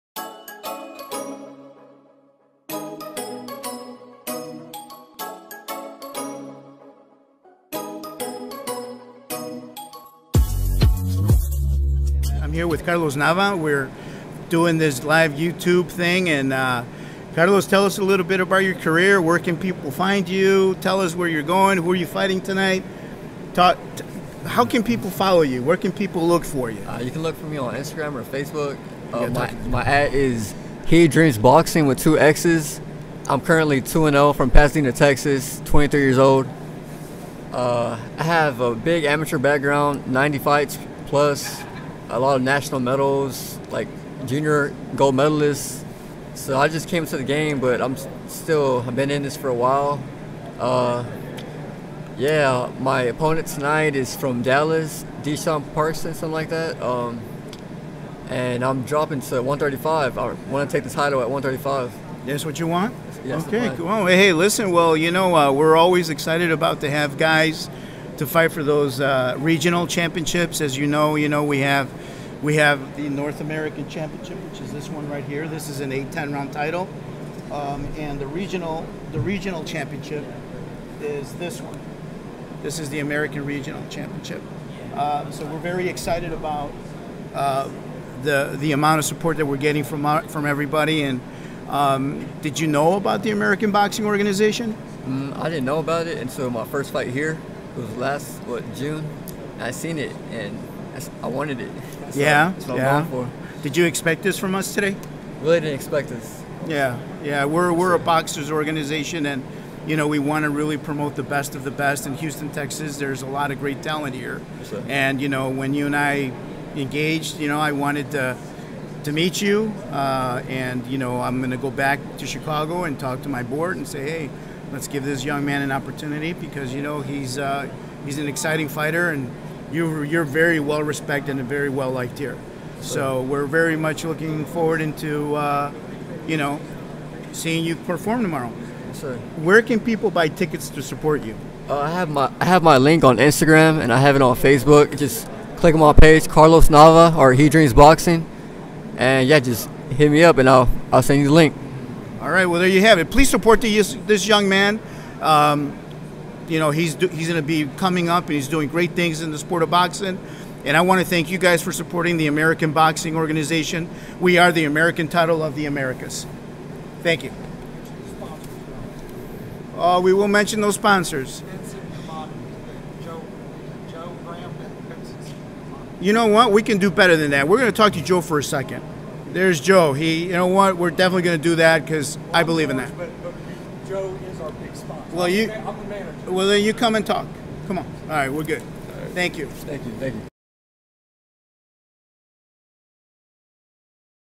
Carlos Nava Professional Boxer, Interview w/CEO, Juan Curiel of the American Boxing Organization
Carlos Nava is a Professional Boxer located in Houston, TX.  Carlos sits down with the CEO of the American Boxing Organization, Juan Curiel to discuss his upcoming fight.